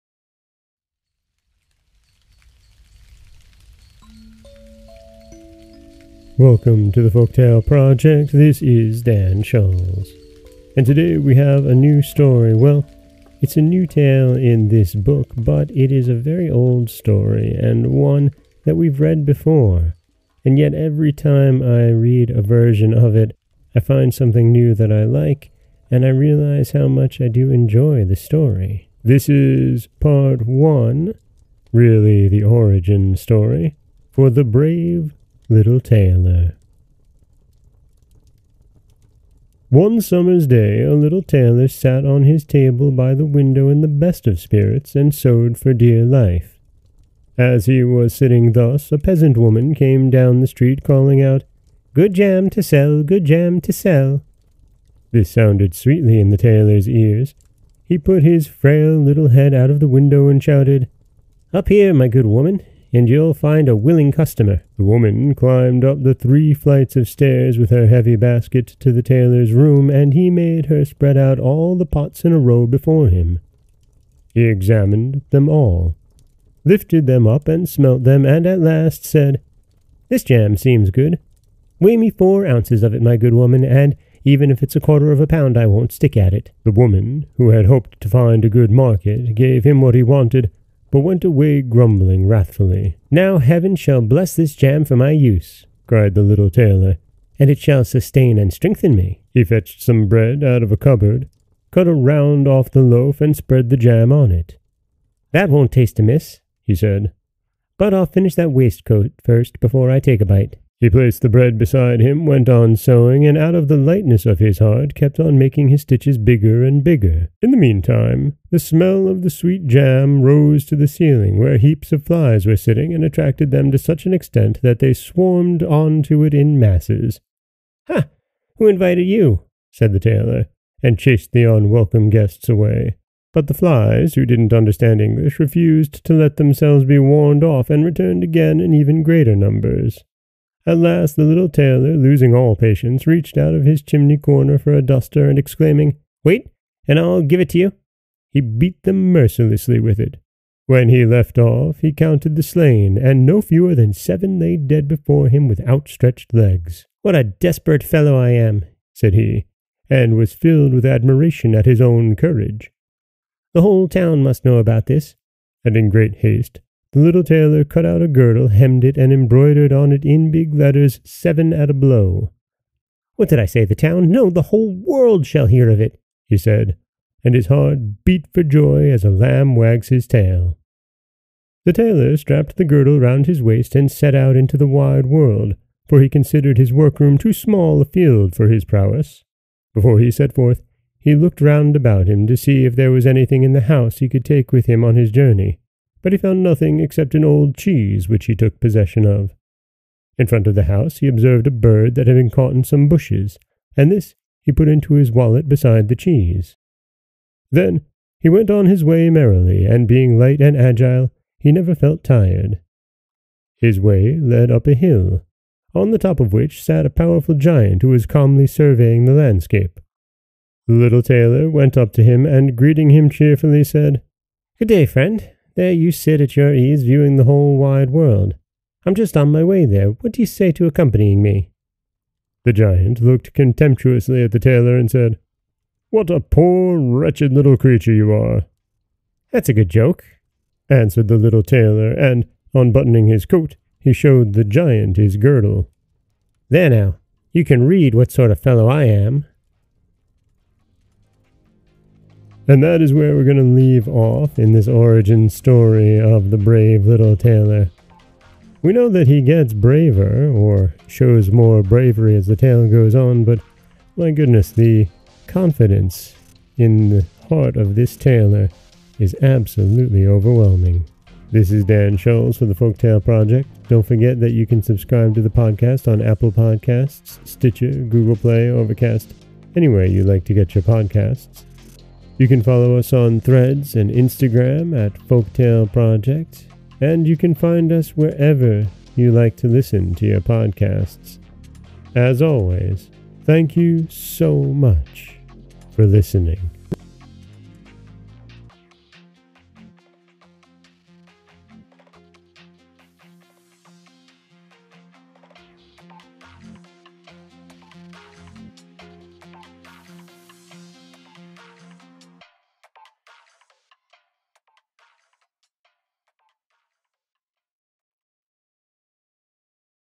The Little Tailor, Pt. 1
Today we begin the tale of 'The Little Tailor', a classic story about a confident young man making his way in the world.

  Book: The Blue Fairy Book by Andrew Lang

 Host: Dan Scholz